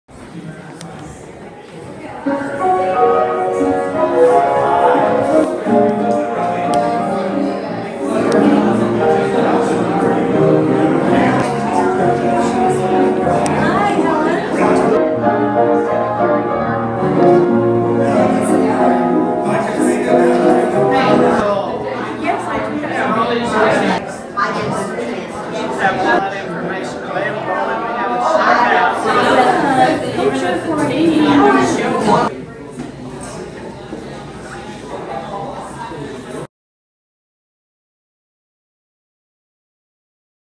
First Look At Symphony Showhouse 2015
Designers prepare to make their proposals.